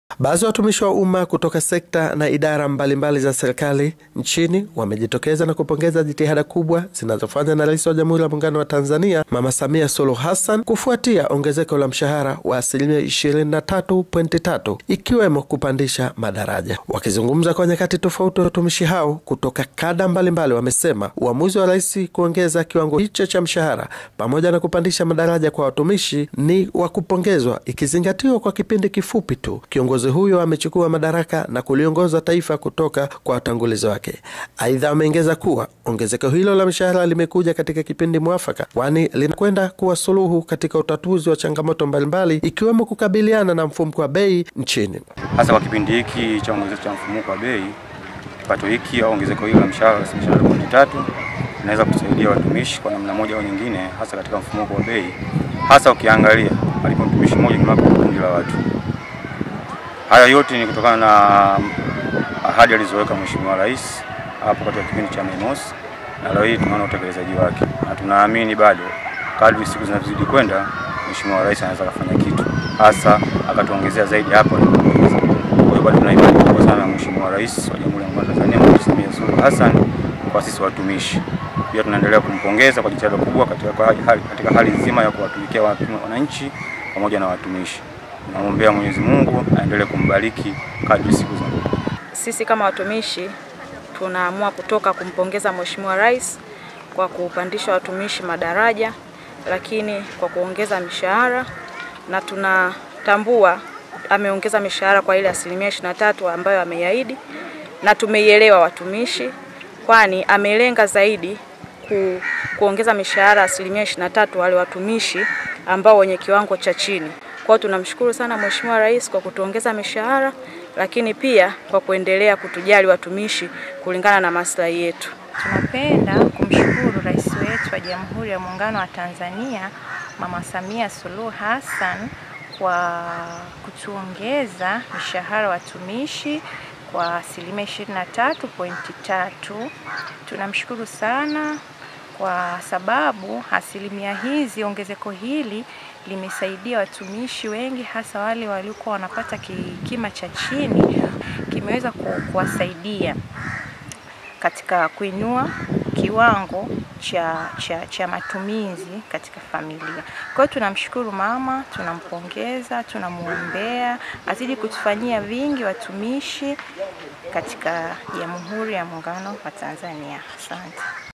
PONGEZI KWA RAIS SAMIA KWA KUONGEZA MISHAHARA
PONGEZI KWA RAIS SAMIA KWA KUONGEZA MISHAHARA KWA WATUMISHI WA UMMA